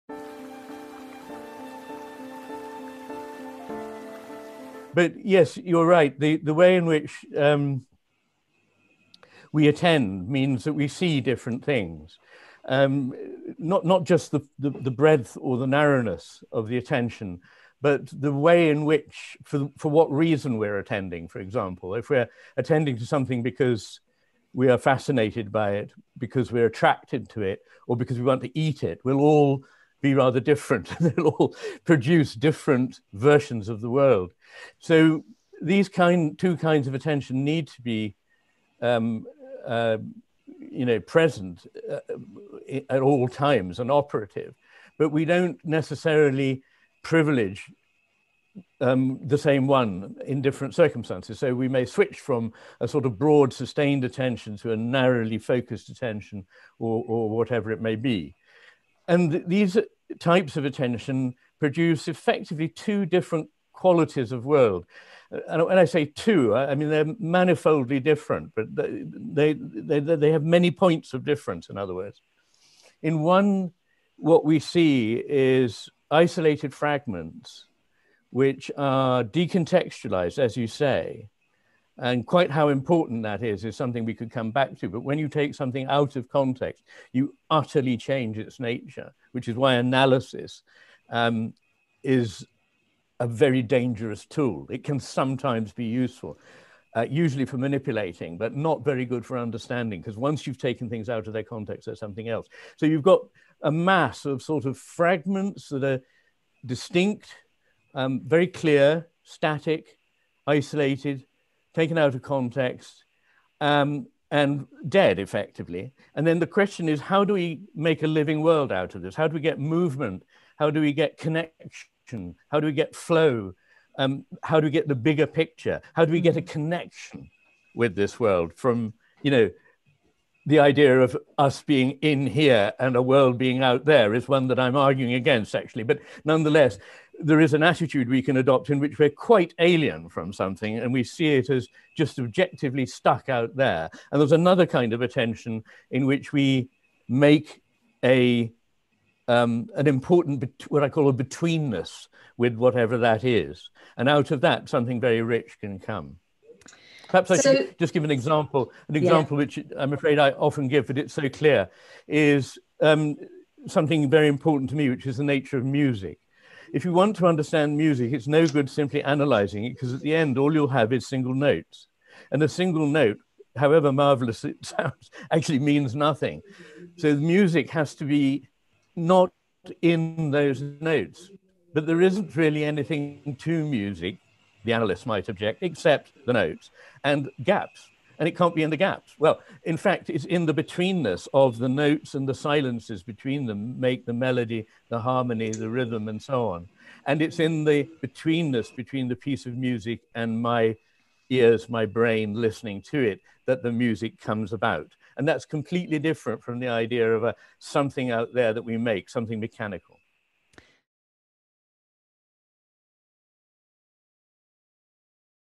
On betweenness, music, and how we attend to the world | Dr Iain McGilchrist
'The Master and His Emissary' by Dr Iain McGilchrist: